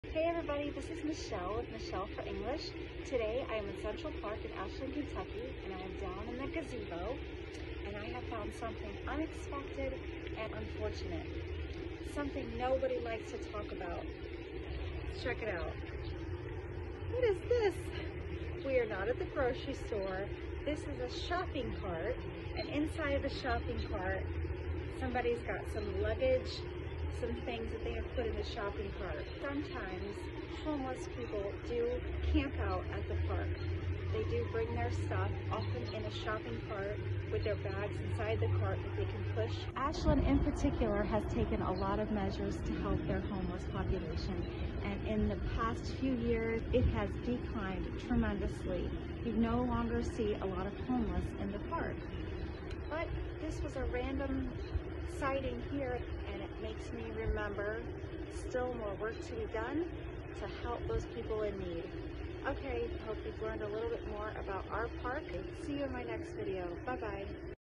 Those in Need: Learn English, Video
✏️ Quiz:
Fill in the blank. Use each answer only one time.

Homeless
Declined
In need
Unfortunate
Work
Camp out
Shopping cart

1. Today I found something ____ that we don’t like to talk about: a ____ ____ filled with somebody’s luggage.
2. Sometimes ____ people use the shopping cart for their things when they ____ ____ at the park
3. The city has helped the homeless population a lot in the past years and the number has ____ or gone down.
4. This sighting reminds me that there is still more ____ to be done to help those people who are ____ ____.

Answers ⬇️

1. Today I found something ____ (unfortunate) that we don’t like to talk about: a ____ ____ (shopping cart) filled with somebody’s luggage.
2. Sometimes ____ (homeless) people use the shopping cart for their things when they ____ ____ (camp out) at the park
3. The city has helped the homeless population a lot in the past years and the number has ____ (declined) or gone down.
4. This sighting reminds me that there is still more ____ (work) to be done to help those people who are ____ ____ (in need).

🗣 Video Prompts:

🛒 Does your town or city also have homeless who camp out in the parks or on the streets?

💛 What does your town or city do to help people in need?